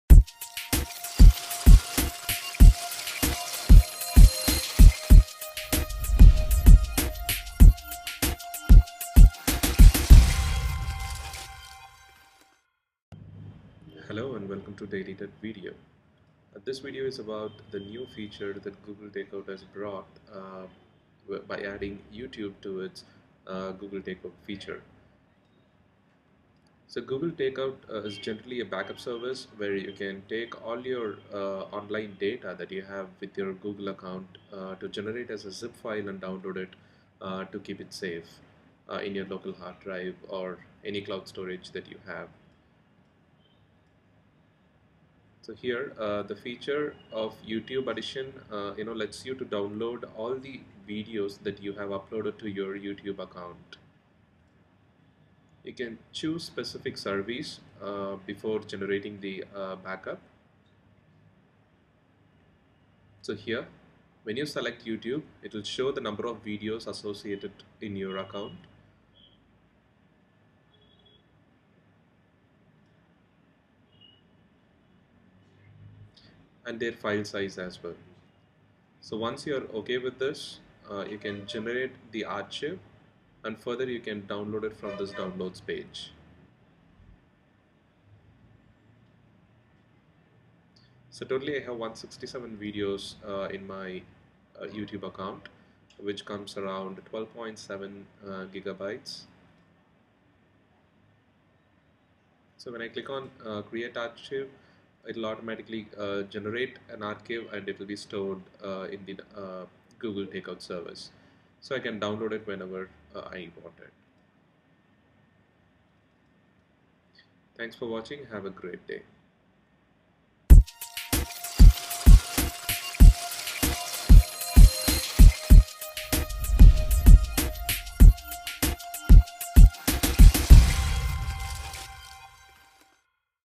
Download Youtube Videos via Google Takeout
You can now download all your Youtube videos via Google Takeout now if you want to delete your google account.